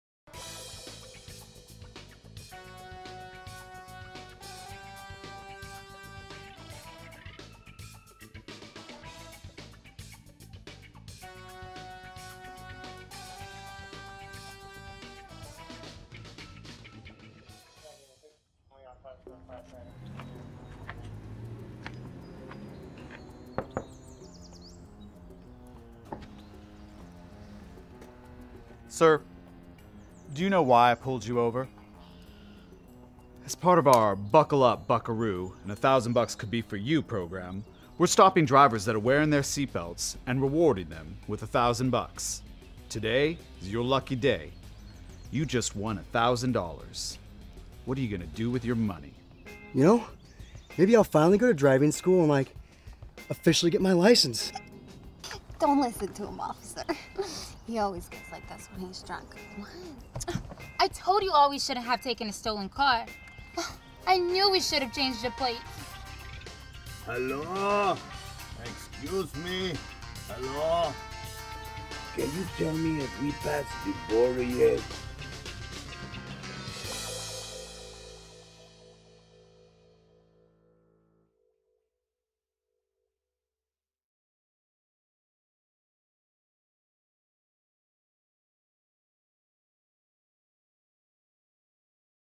Buckle Up, Buckaroo
The Buckle Up, Buckaroo and a 1000 Bucks Could Be For You Program started in April of 2017 thanks to voters' overwhelming approval of Proposition DCOH! Within a short amount of time, voters have already been rewarded with a program that has yielded rich results.  Just another example of your tax dollars hard at work!